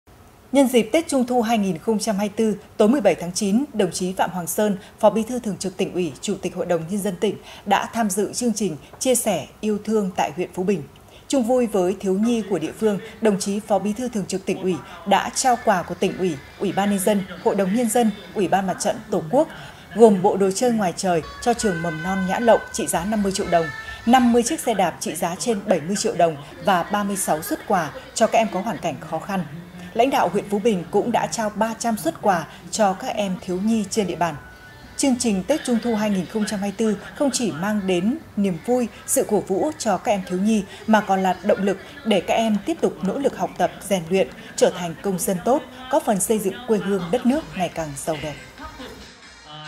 Đồng chí Phó Bí thư Thường trực Tỉnh ủy tặng quà Trung thu tại huyện Phú Bình | Thái Nguyên TV
Nhân dịp Tết Trung thu 2024 , tối 17/9, đồng chí Phạm Hoàng Sơn, Phó Bí thư Thường trực Tỉnh ủy, Chủ tịch HĐND tỉnh đã tham dự chương trình chia sẻ yêu thương tại huyện Phú Bình.